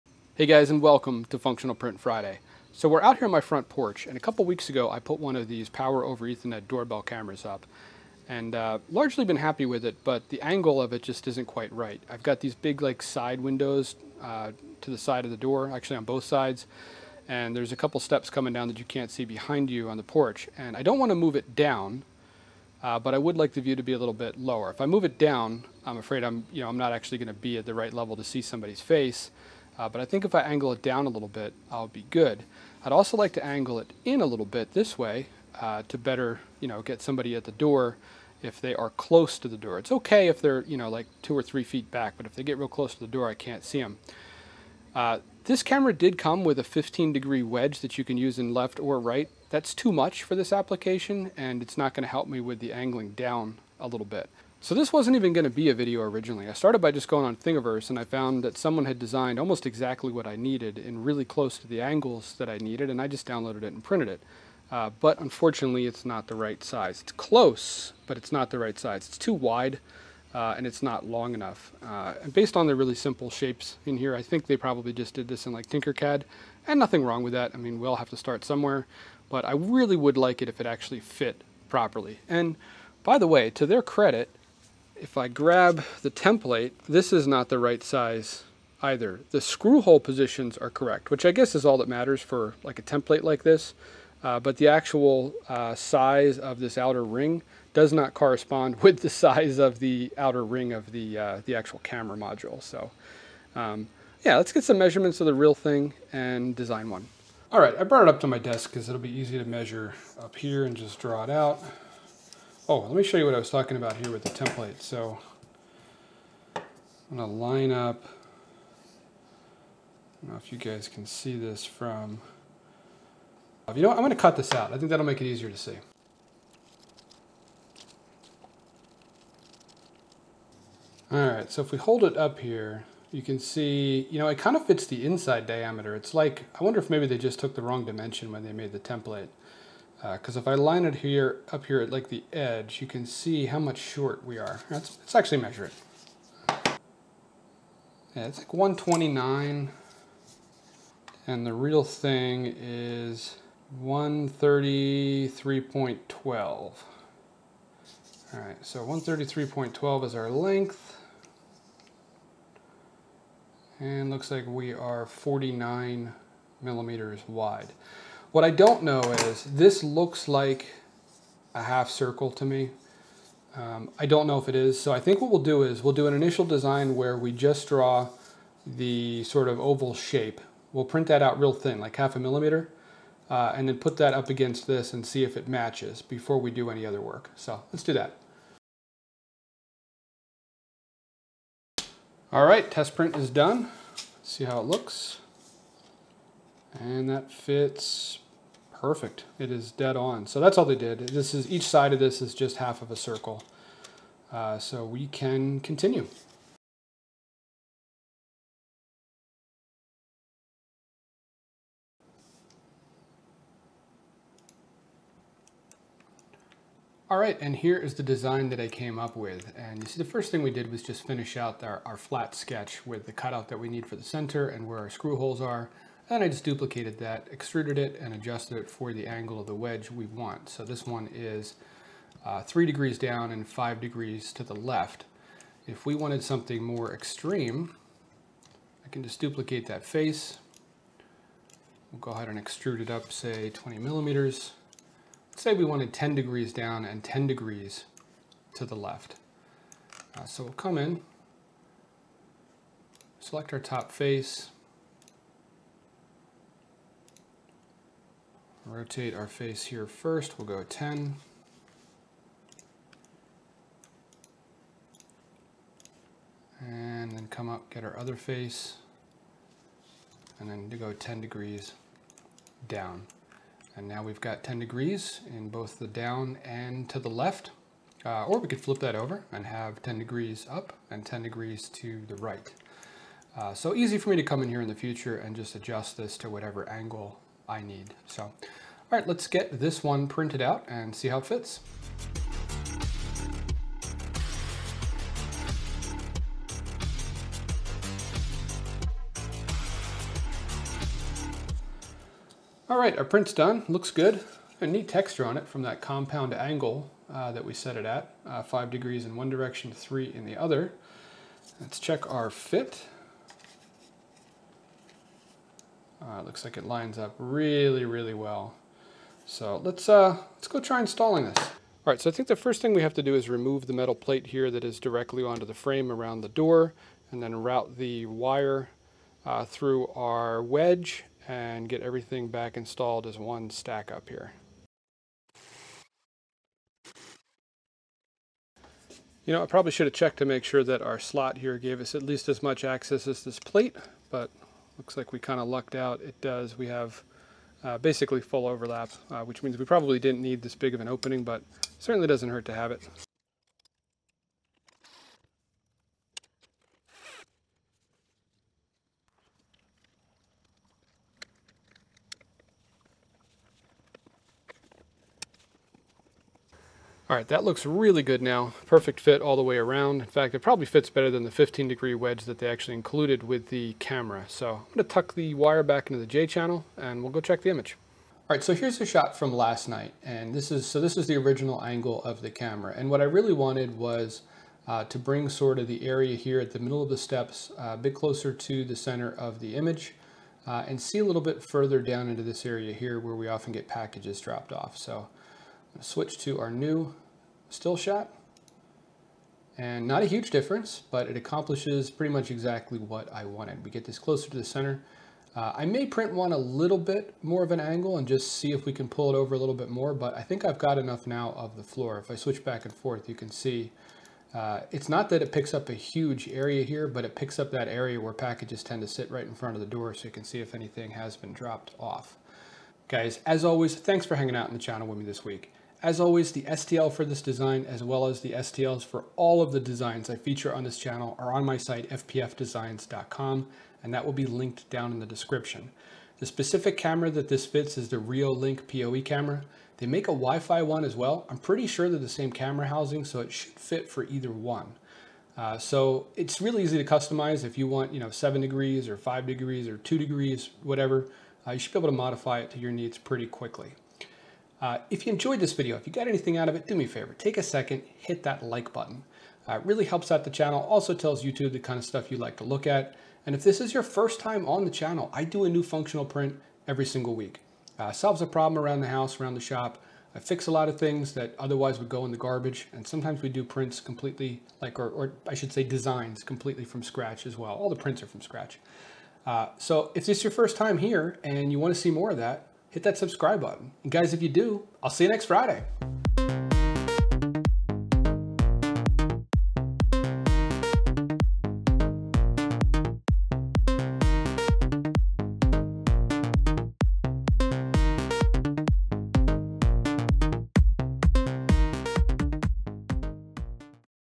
3D Printed Doorbell Cam Angled Wedge for Reolink
Black PLA
REOLINK POE Cam
REOLINK WiFi Cam

New videos published every Friday, featuring a new 3D printed functional object, how I use it, and design considerations.

POE
wireless
wifi
custom
ipcam
functional print